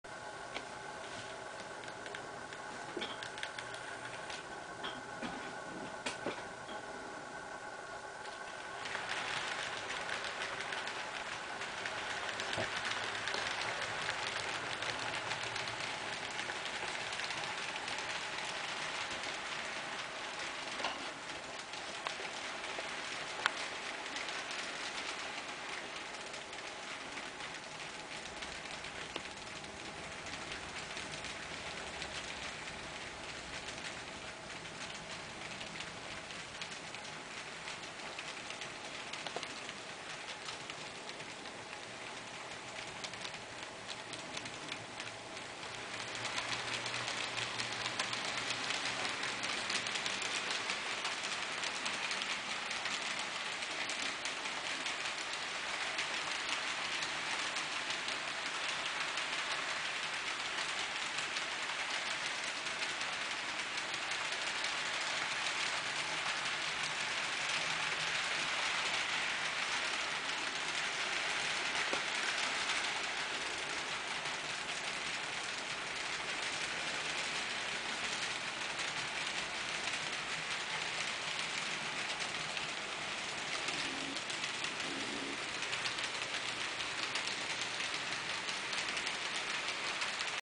Randy Gustafson N scale GE 70-Tonner pulls Kato Daylight
I hosted Randy Gustafson's N scale GE 70-Tonner. Resin kit, on Tomy-Tec mechanism, one traction tire.  Watch Randy's little loco pull an entire 12-car Kato Daylight train around the layout!